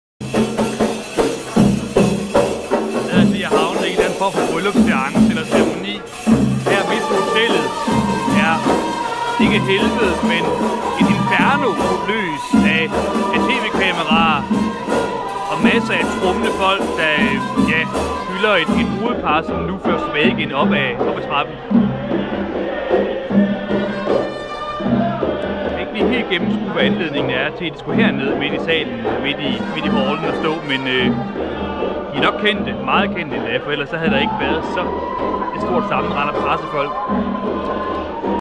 Bryllup i Cairo
Et kendt brudepar bestormes af tv-folk til deres bryllup. Hotel Le Meridian